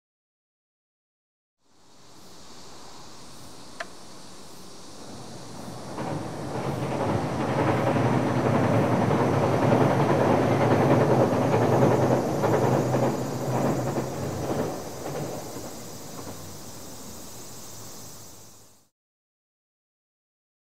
2700系 特急南風10号 第一吉野川橋梁を行く[4K60P]
第一吉野川橋梁を行く2700系 特急南風10号